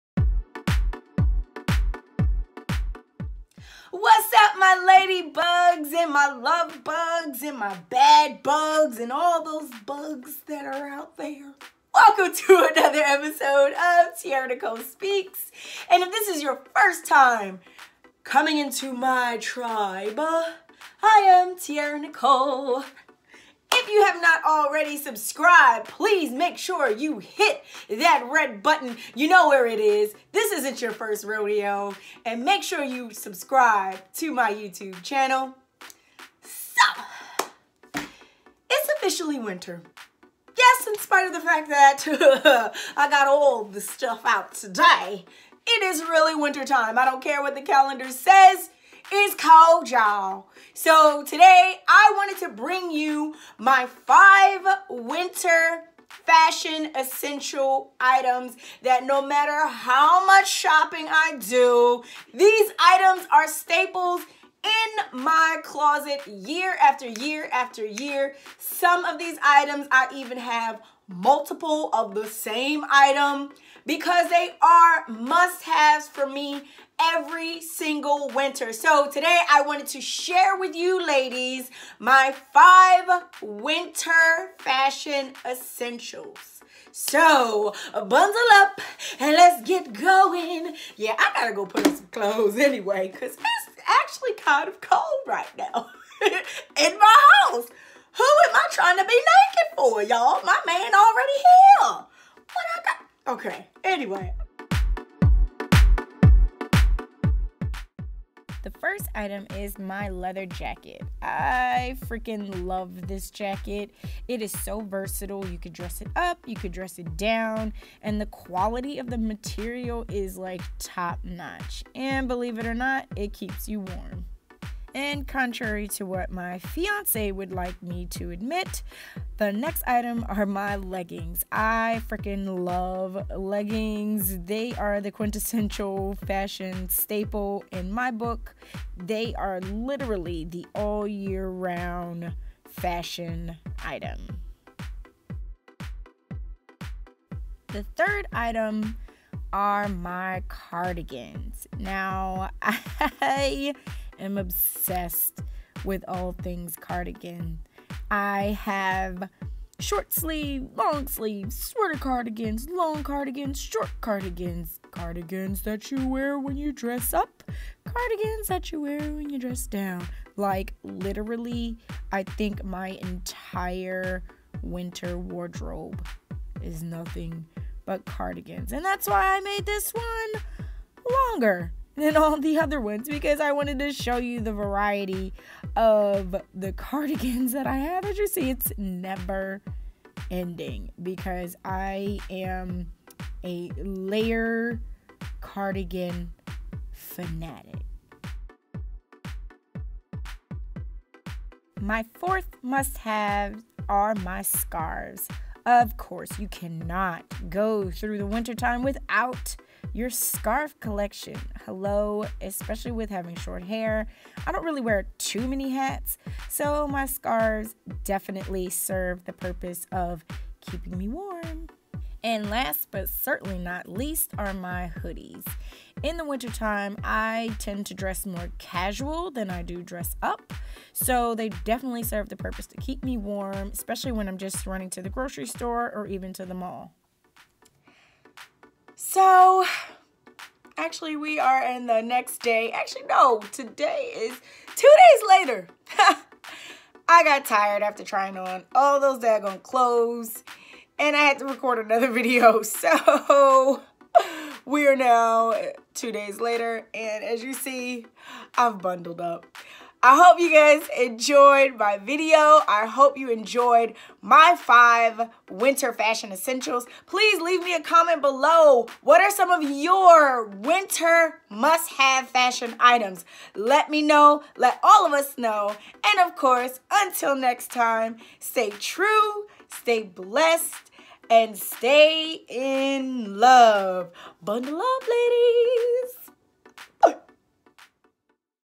MY 5 WINTER FASHION ESSENTIALS
I don't care what the calendar says, ITS WINTER!

Hey boo thangs!

Winter is here so its def time to pull out all of my usual winter essentials!  Check out my 5 winter fashion essentials that I can NOT go a season without!!!

I love you all so much and am grateful for your love and support!!